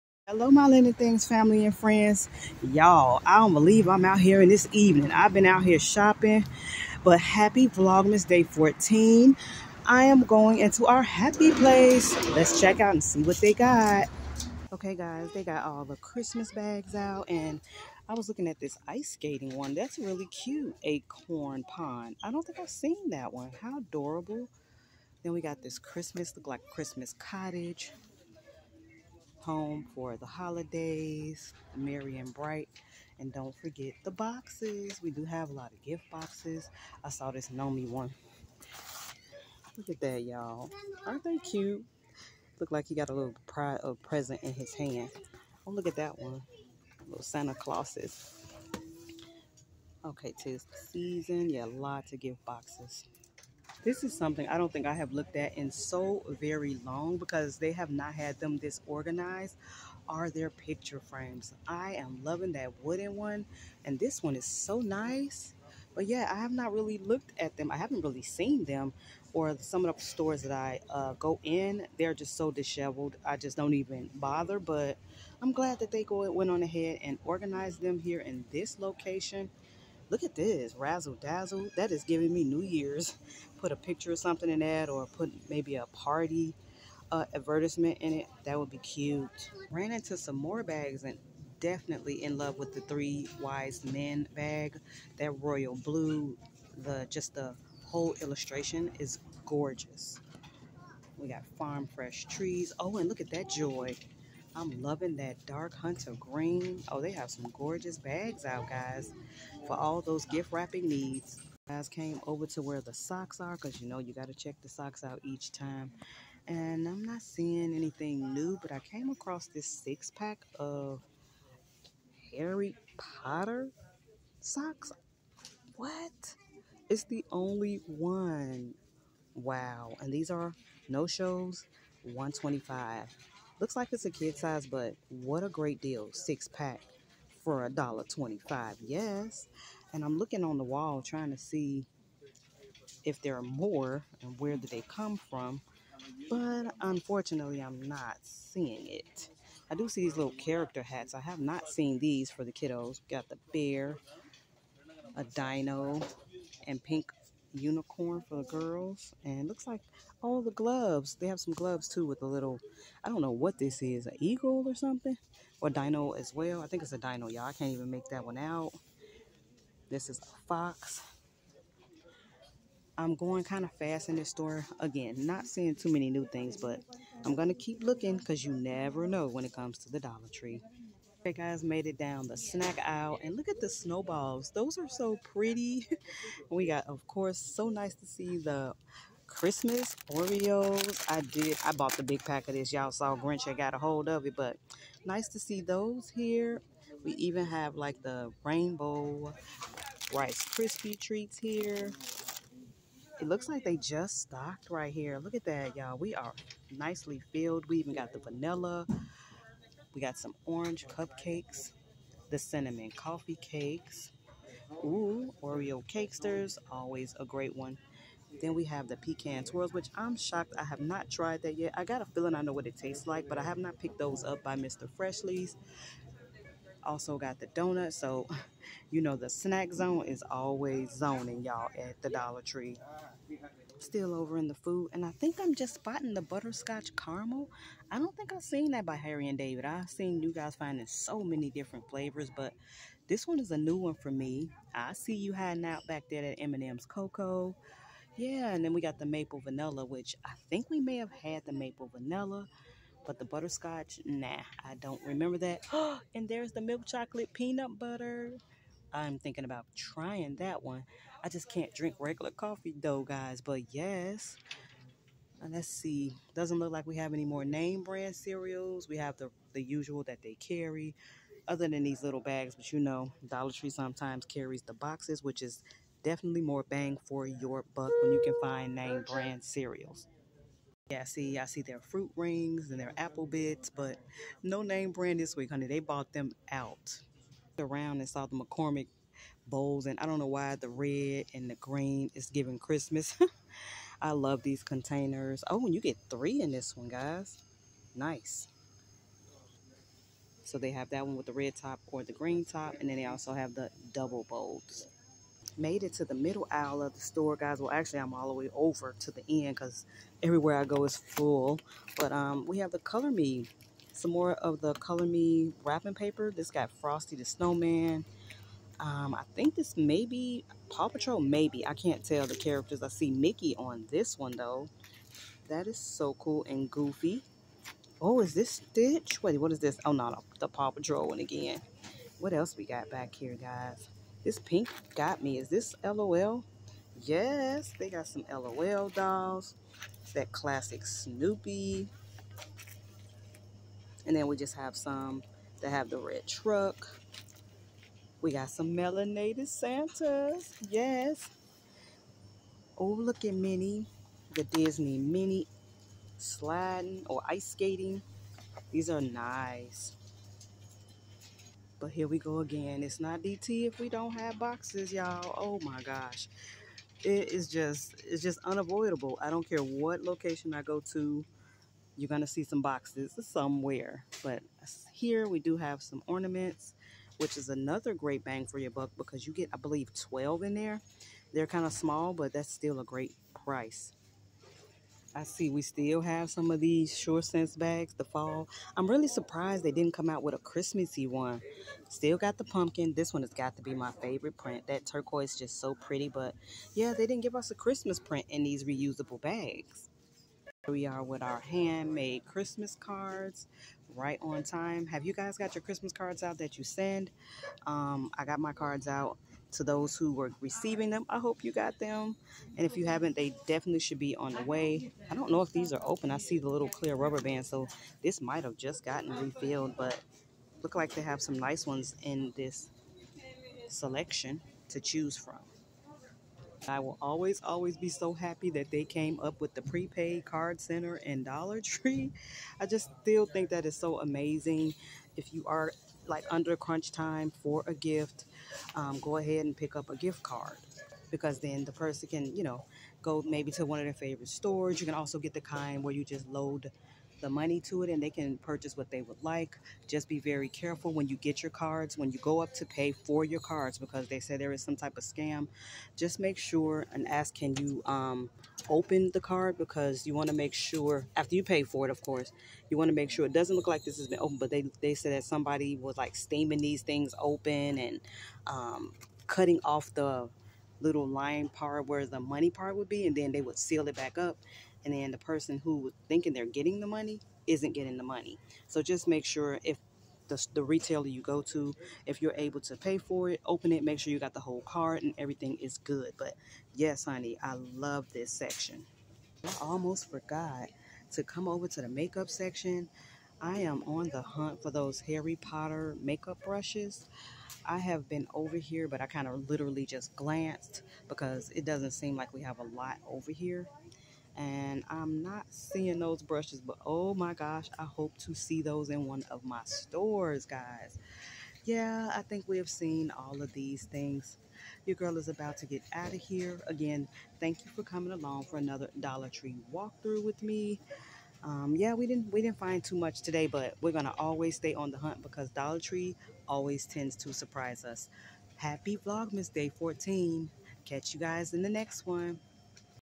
COME WITH ME TO DOLLAR TREE 12/14/23 VLOGMAS DAY 14
Come with me and let's see what treasures we find on this trip to our Happy Place 😍